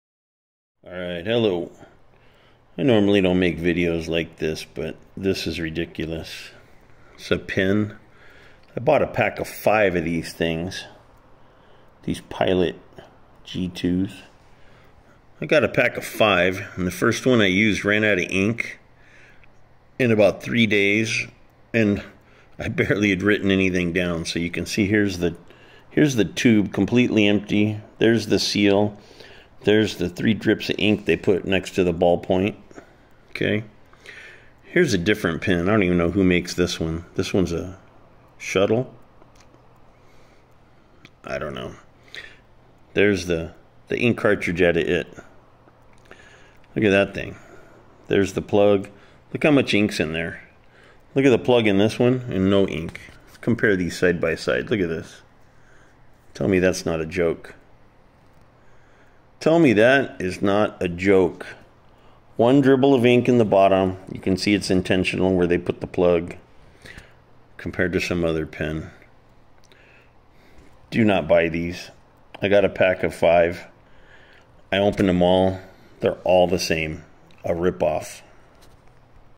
Pilot G2 Pen Ink Rip Off, Don't Waste Your Money on These!
Pilot G2 Ball Point  Pen Ink is a Total Rip Off, Don't Waste Your Money on These!

and then look at my pen from 1 week ago, almost no ink.

Clearly they keep putting less and less